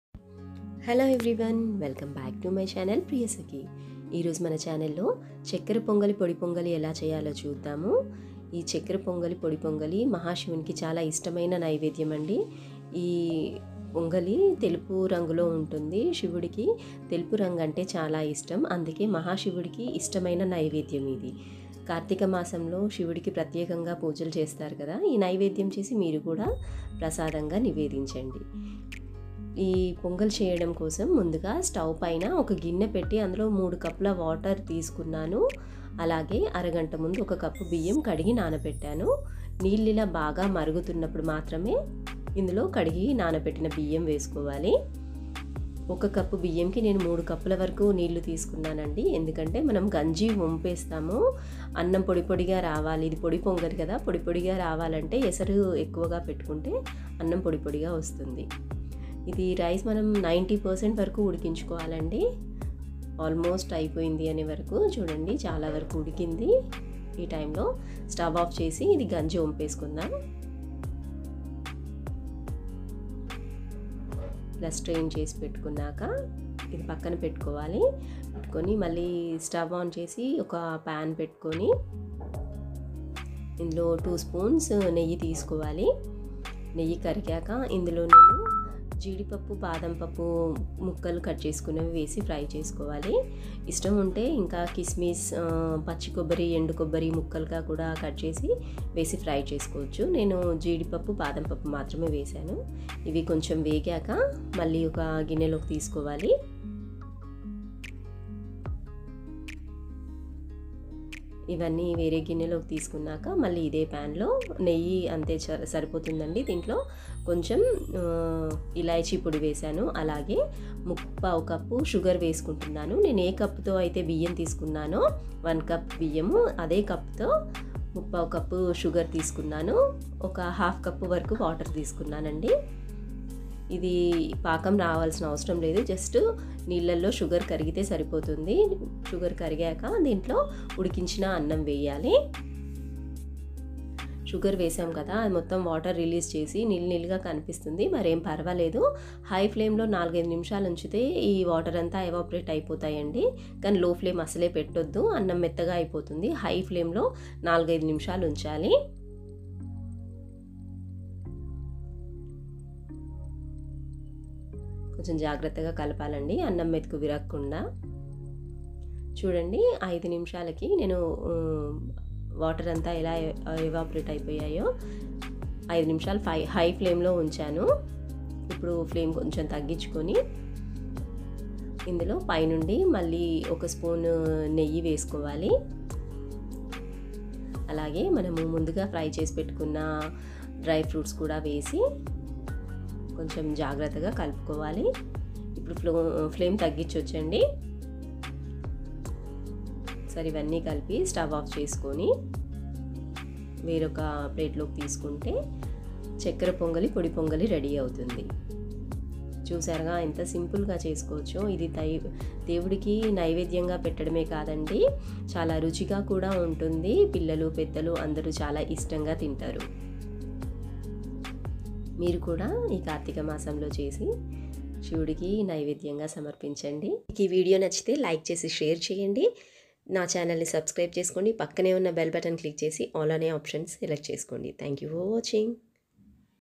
చెక్కర పొంగలి పొడి పొంగలి
చెక్కర పొంగలి పొడి పొంగలి/ మహా శివునికి ఇష్టమైన పదార్థం చెక్కర పొంగలి పొడి పొంగలి/ఈ పొంగలి చాలా తొందరగా చేసుకోవడమే కాకుండా ఎంతో రుచిగా  కూడా ఉంటుంది